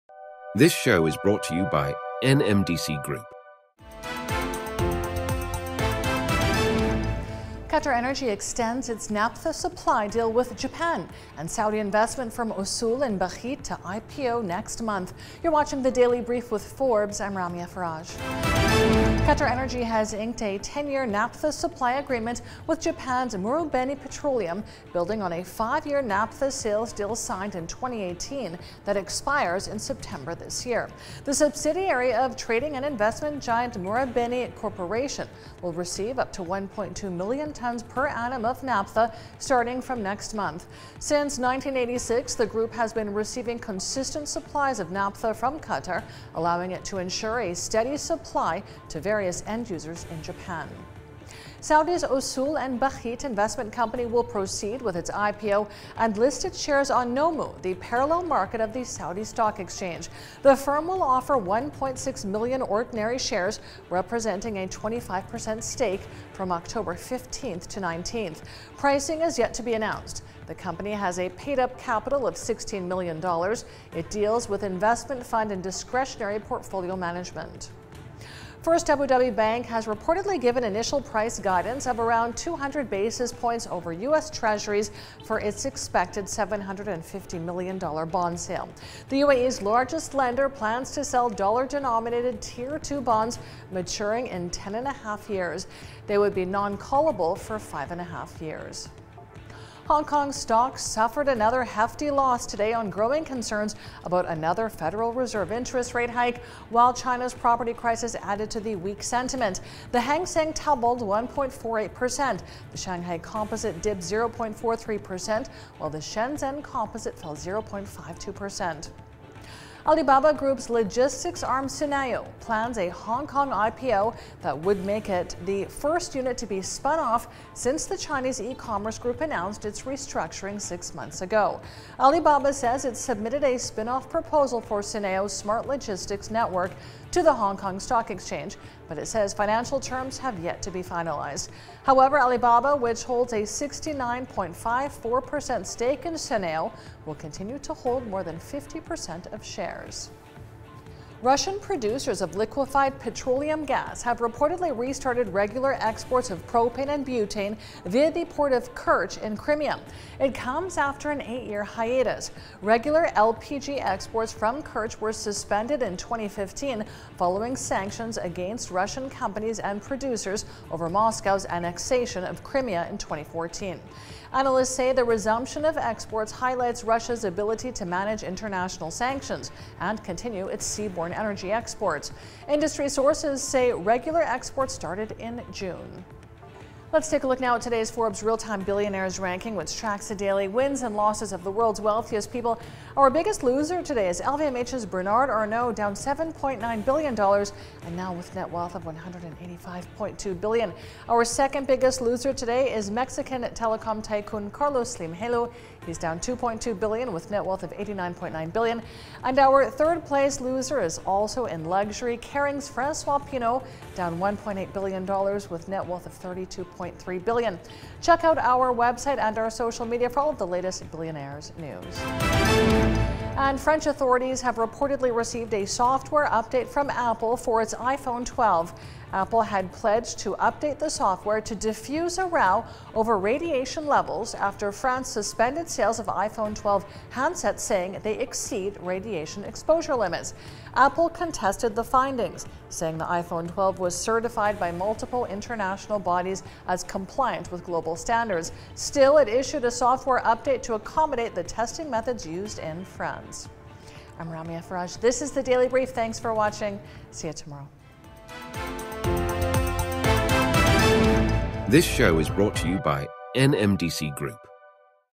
Evening Brief 26-09-2023 | QatarEnergy extends naphtha supply deal with Japan
QatarEnergy extends naphtha supply deal with Japan & Saudi investment firm Osool & Bakheet to launch IPO in October, on the DailyBrief with @RamiaFarrage